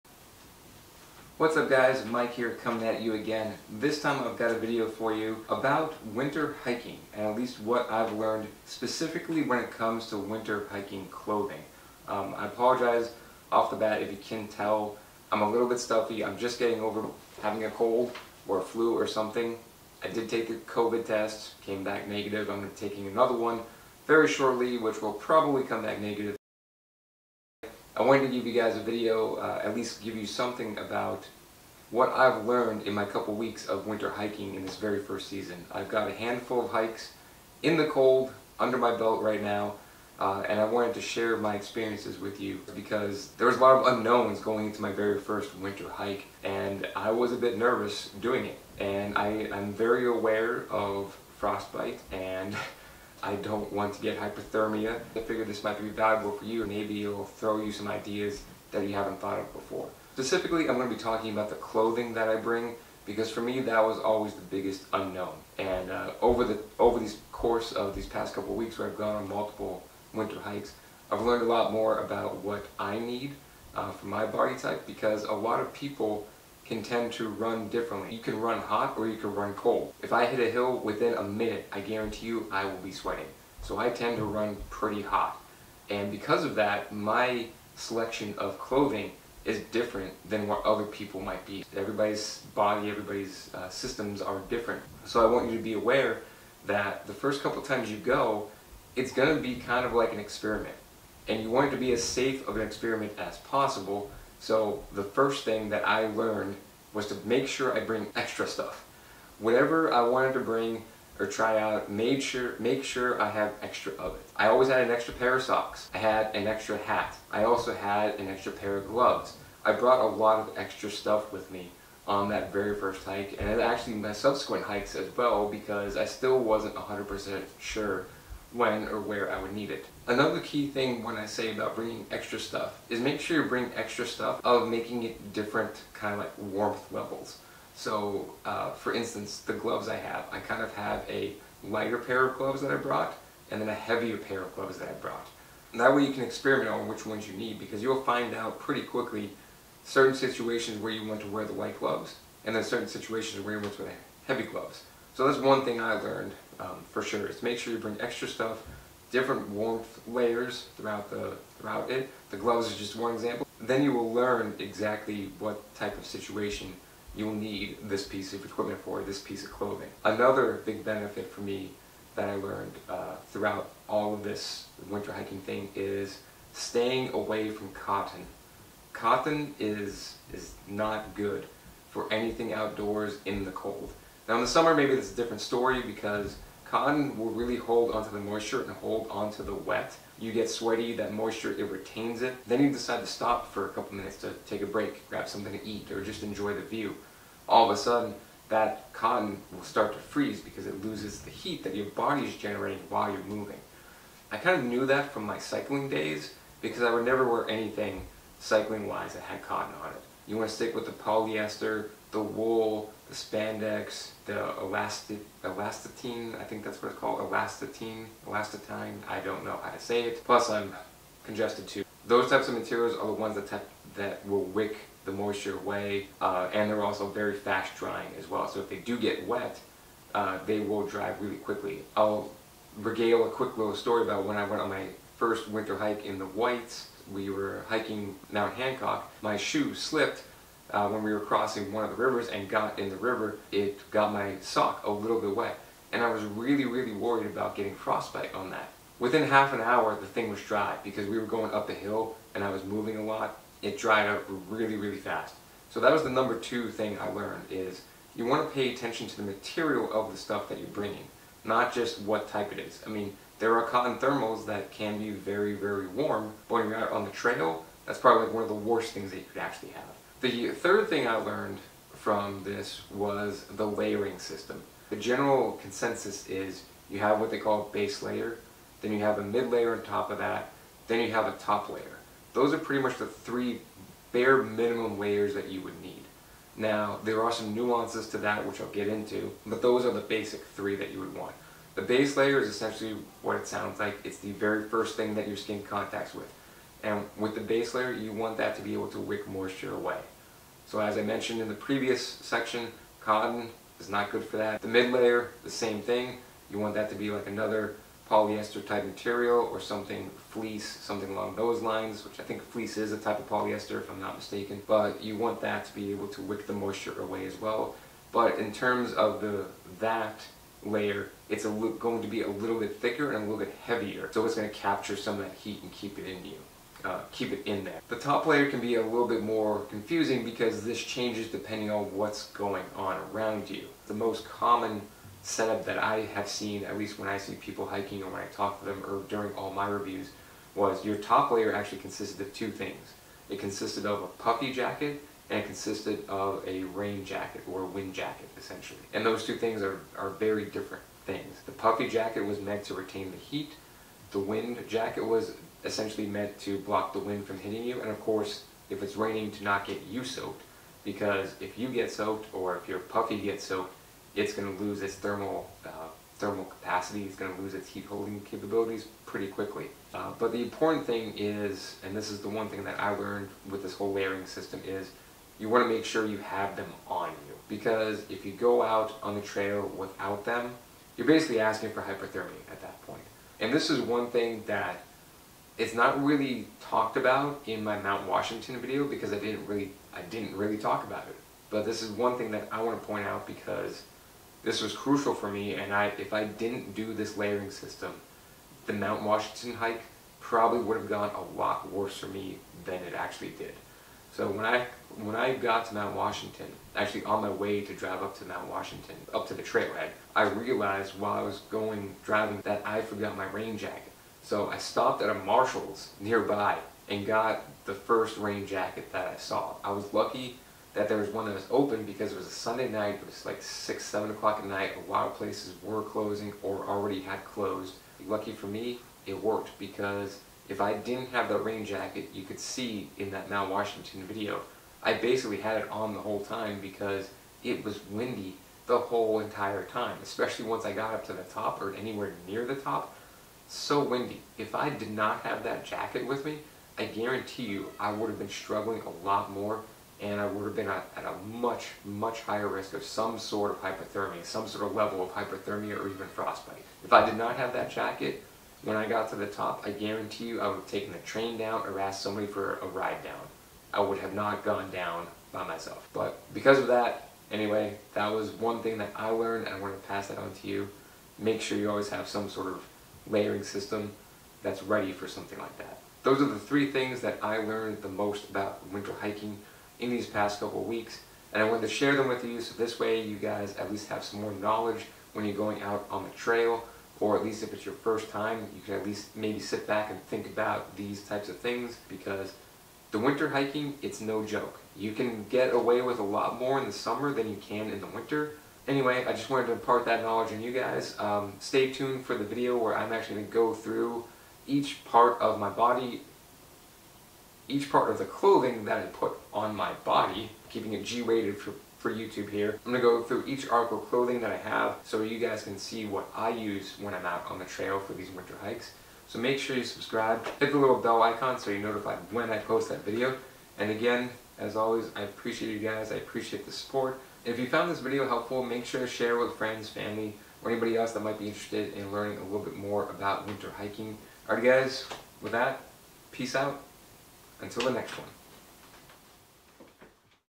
Three things you NEED TO KNOW OR DO if you are even THINKING about hiking in the winter!
Today I have for a you a video about things I have learned while I started this journey of winter hiking. If you don't know; this is my first real season hiking in wintery weather and I am still learning but I wanted to share with you three things that I did learn (and I am sure there is plenty more!).

The reason I am posting this is because there has been a lot of sad news lately this season when it comes to hikers getting hurt or even dying on the trails. Winter hiking is no joke!

So make sure you watch to the end for the last one as that might be the most important!

And if you think I missed anything or if you want me to elaborate on something please let me know.

Cheers all!